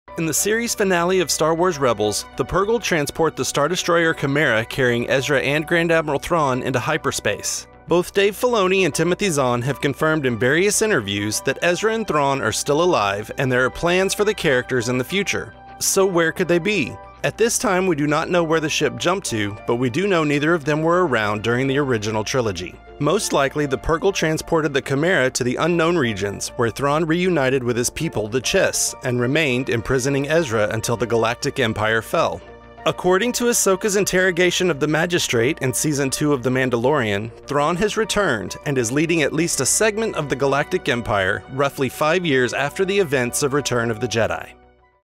Where Are Ezra And Grand Admiral Thrawn? Star Wars
In the series finale of Star Wars: Rebels, the Purgill transport the Star Destroyer Chimaera - carrying Ezra and Grand Admiral Thrawn - into hyperspace.

Both Dave Filoni and Timothy Zahn have confirmed in various interviews that Ezra and Thrawn are still alive and there are plans for the characters in the future.

So where could they be?